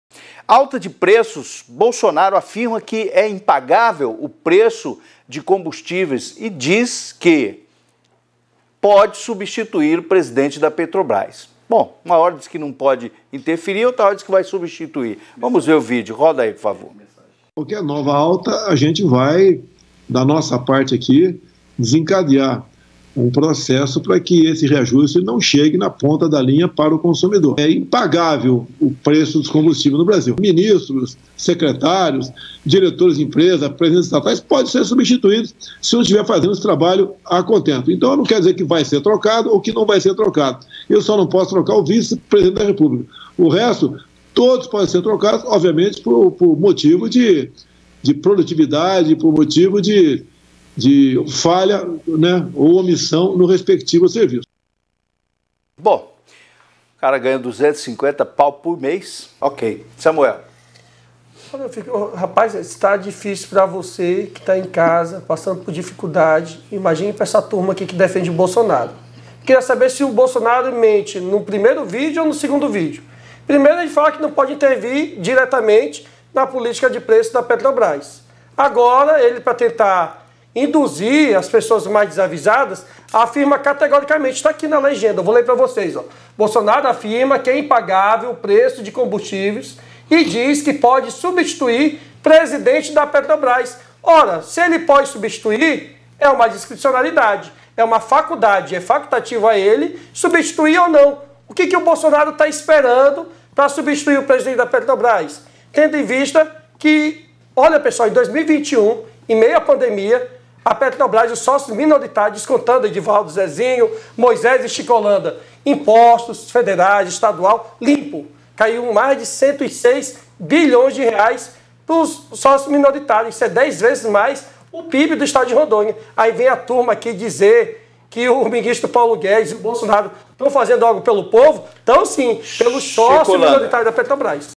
BOLSONARO AFIRMA QUE É IMPAGÁVEL O PREÇO DE COMBUSTÍVEIS E DIZ QUE PODE SUBSTITUIR
BOLSONARO AFIRMA QUE É IMPAGÁVEL O PREÇO DE COMBUSTÍVEIS E DIZ QUE PODE SUBSTITUIR PRESIDENTE DA PETROBRAS

▶ O QUE VOCÊ VAI ENCONTRAR NESTE VÍDEO:

0:16 – Introdução
0:17 – Fala de Bolsonaro
0:56 – Comentário Edvaldo Soares
1:00 – Comentário Samuel Costa